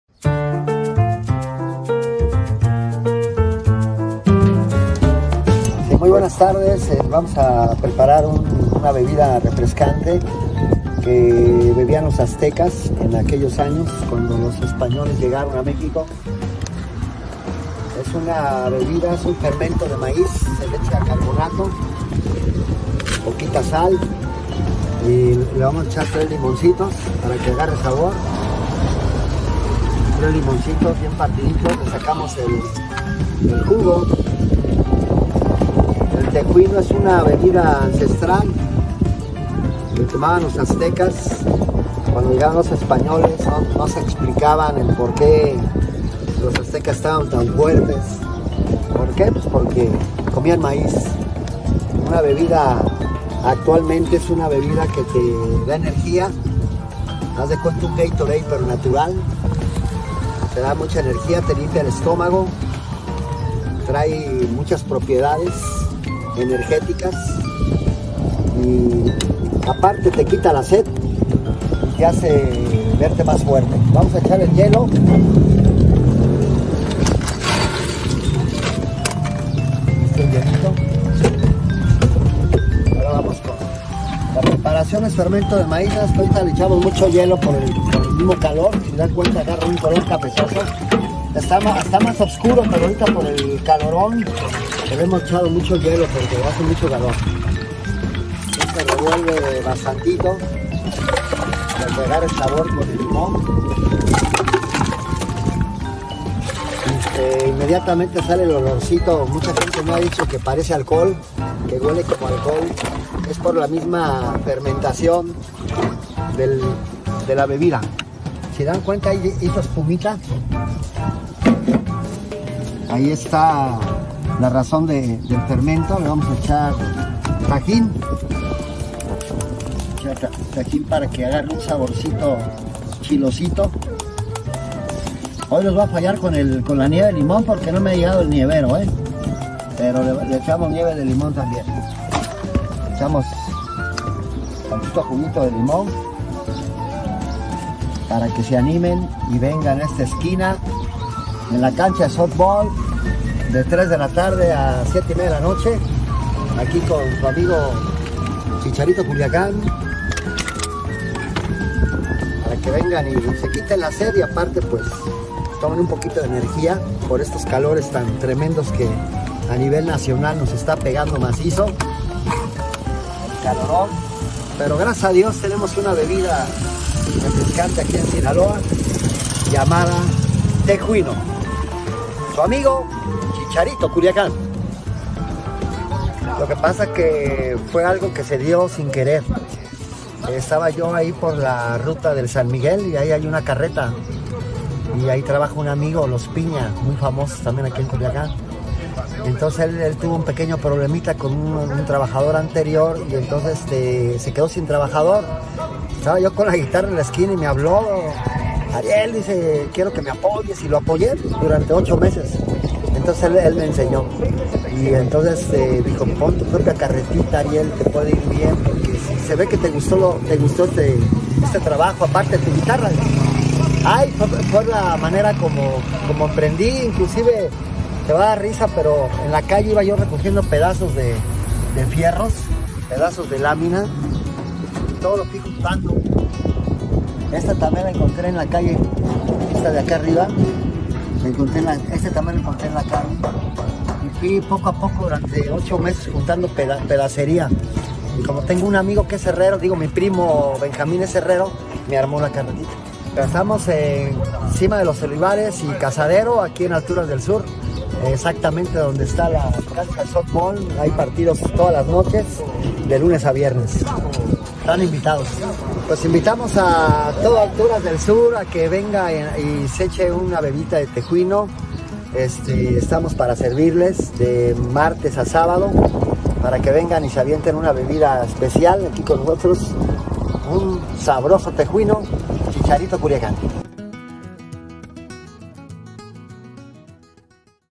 Fuimos a Alturas del Sur a probar los Tejuinos de Chicharito Culiacán
En las inmediaciones del Parque Fiu Fiu en Alturas del Sur, justo en el Campo de Softbol, un nuevo emprendimiento ha tomado forma de la mano de Ariel Ibarra Ojeda, mejor conocido por todos en Culiacán como Chicharito.

Chicharito es un ejemplo de esfuerzo y perseverancia. Con su característico carisma y una bebida que promete revitalizar a quien la pruebe, invita a todos los vecinos de Alturas del Sur a que se acerquen y disfruten de un sabroso tejuino.